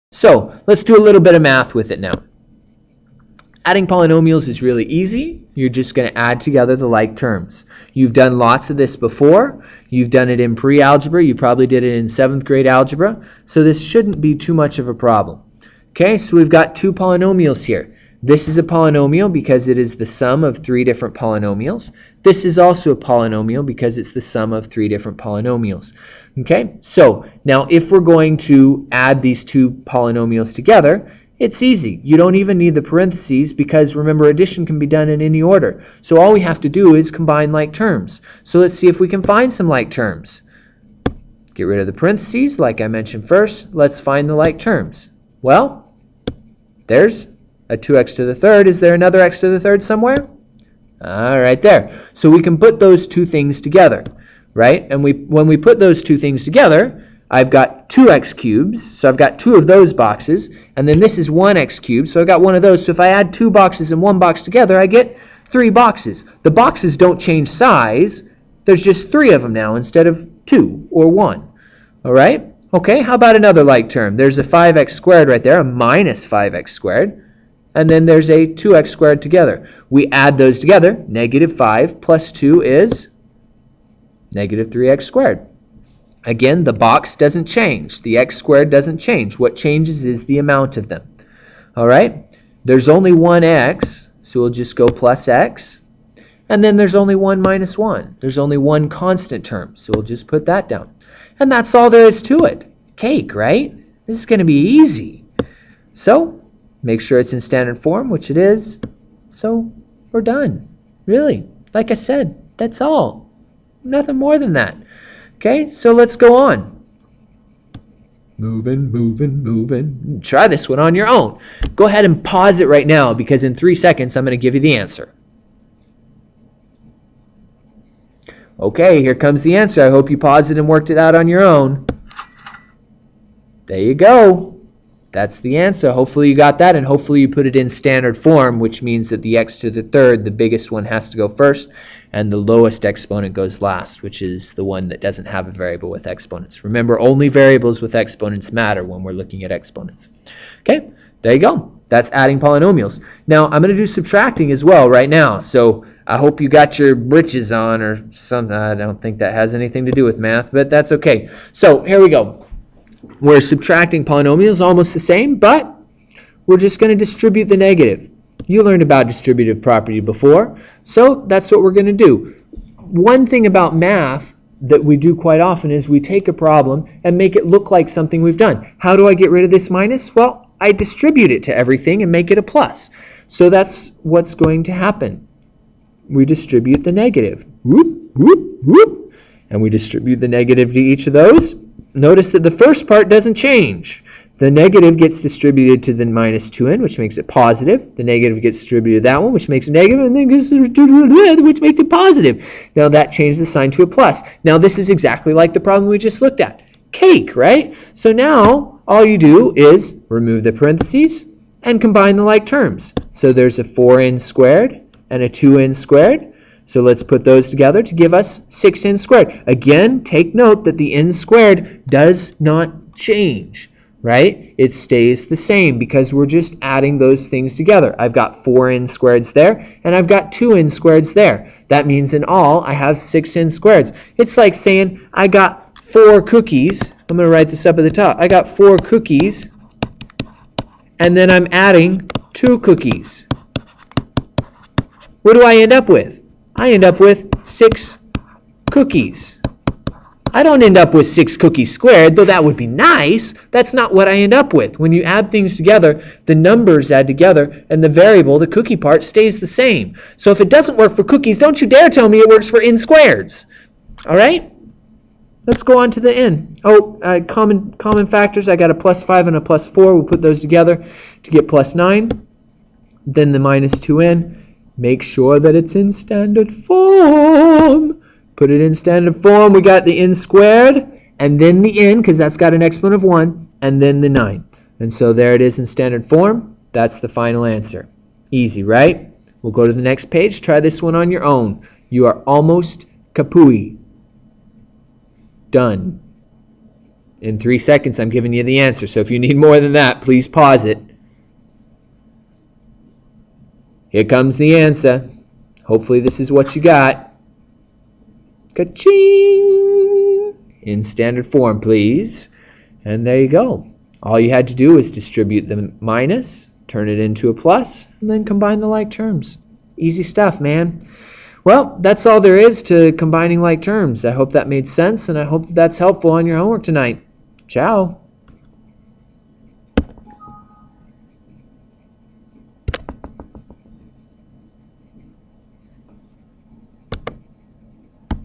5 Minute Math: Like Terms 4 (adding and subtracting polynomials)
One example and one practice problem (with answer) for both adding and subtracting polynomials.